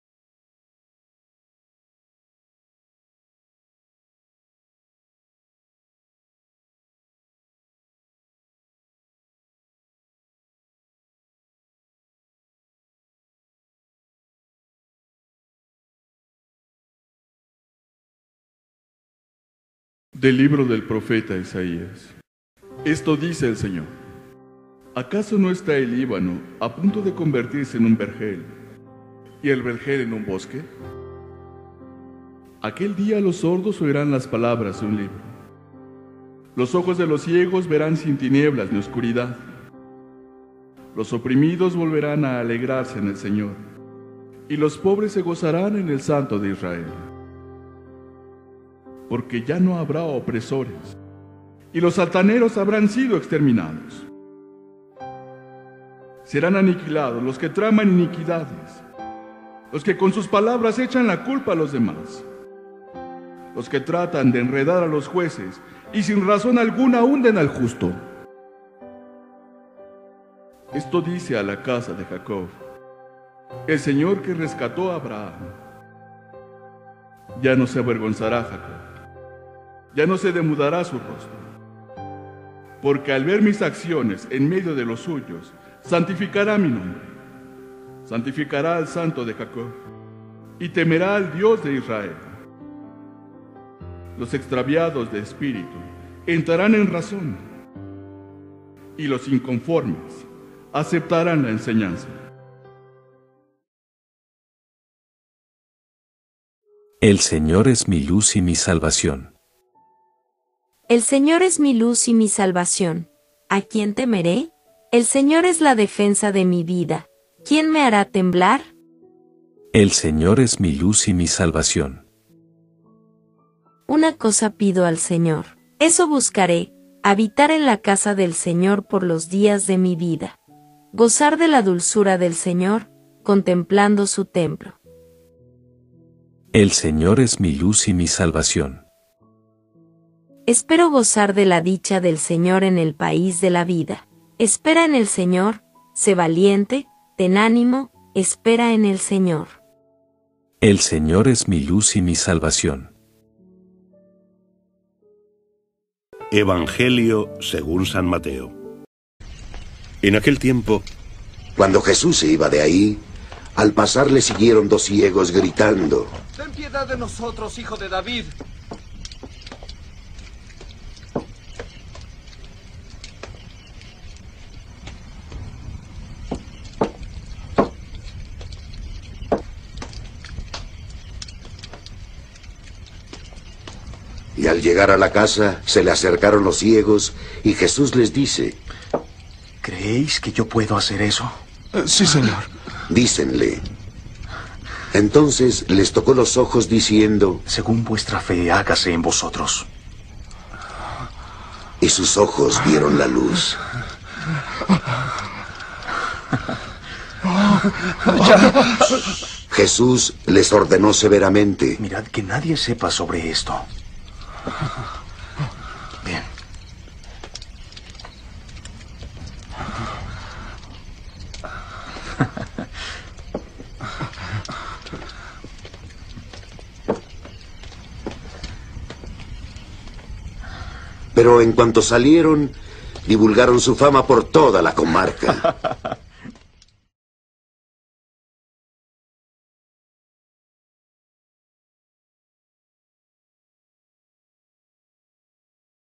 EVANGELIO VIERNES 03/12/2021 | MATEO 9,27-31 VIERNES I SEMANA DE ADVIENTO
LECTURAS Y EVANGELIO EUCARISTÍA VIERNES DICIEMBRE 3 DE 2021 VIERNES I SEMANA DEL TIEMPO DE ADVIENTO CICLO C Primera Lectura: Del libro del profeta Isaías 29,17-24 Salmo 26,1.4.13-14 R/. El Señor es mi luz y mi salvación. Evangelio según San Mateo 9,27-31